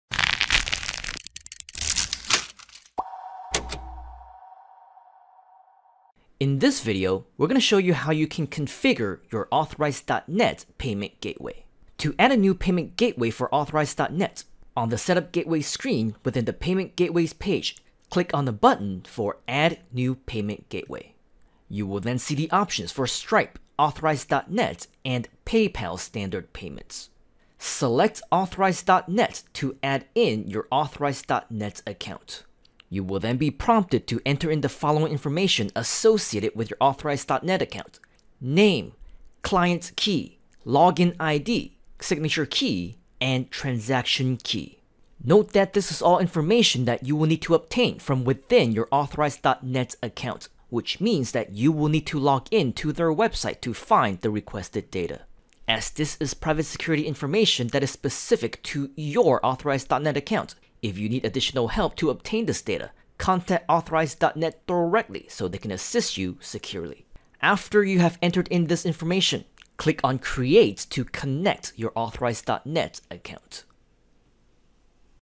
[GrooveFunnels WalkThrough] GrooveSell: Products (Gateways - Add New Authorize.net Payment Gateway)
To add a new payment gateway for Authorize.net, on the Setup Gateways screen within the Payment Gateways page, click on the button for "+Add New Payment Gateway."

You will see the options for Stripe, Authorize.net and Paypal Standard Payments.

Select "Authorize.net" to add in your Authorize.net account.

You will then be prompted to enter in the following information associated with your Authorize.net account:

Name
Client Key
Login ID
Signature Key
Transaction Key

Note that this is all information that you will need to obtain from within your Authorize.net account, which means you will need to log in to their website to find the requested data.

As this is private security information that is specific to your Authorize.net account, if you need additional help to obtain this data, contact Authorize.net directly so they can assist you securely.

After you have entered in this information, click on Create to connect your Authorize.net account.